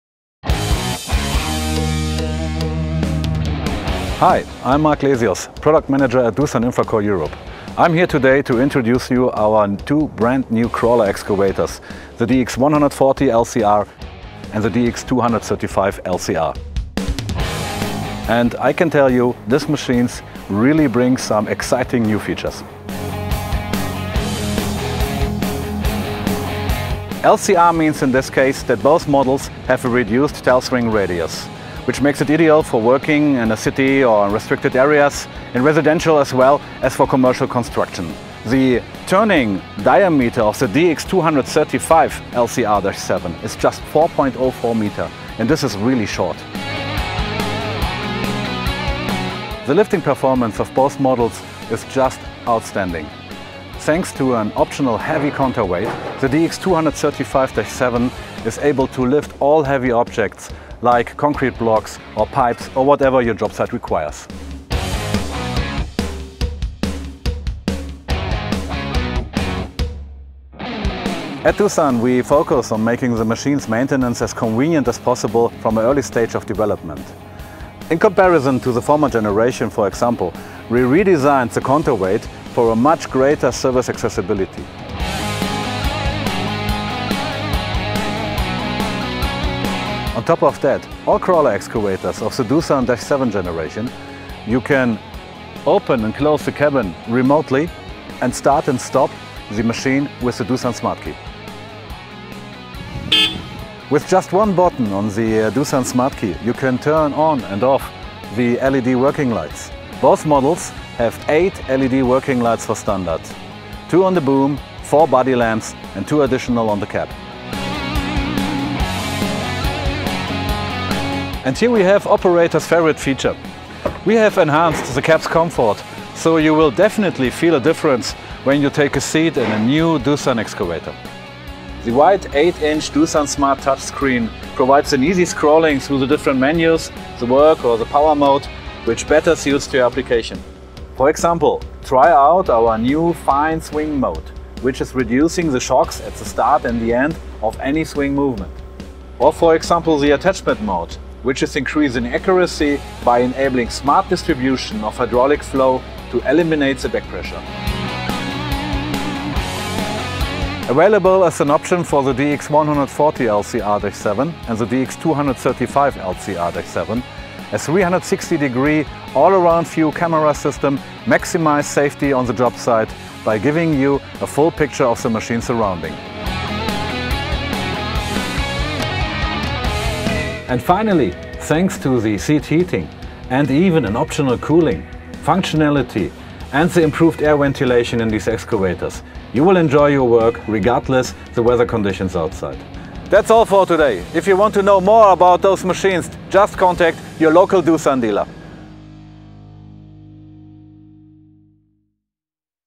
Doosan DX140LCR-7 & 235LCR-7
Lift your business to the next level with the DX140LCR-7 Doosan excavator. The DX140LCR-7 offers the best equilibrium possible between reduced swing radius and stability. One of the shortest swing radiuses in the market with the stability of a conventional excavator.